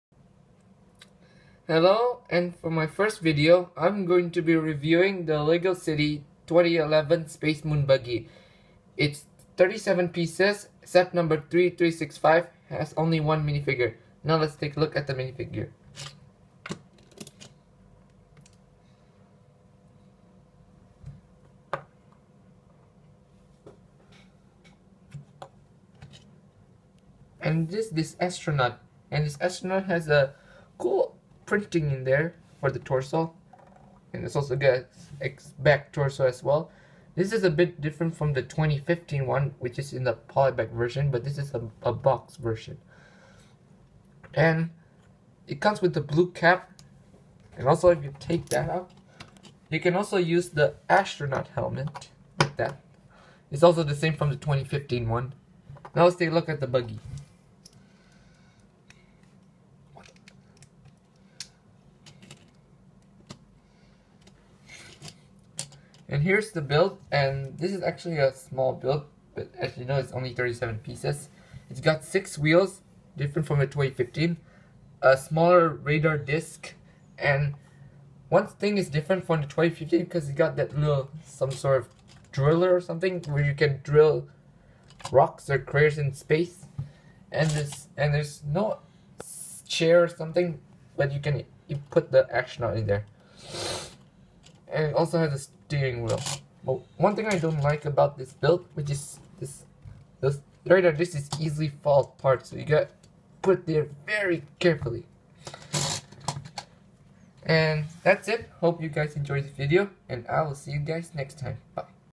Lego City 3365 Space Moon Buggy Review
As the beginning on Youtube, I'll be reviewing the Lego City 2010 Space Moon Buggy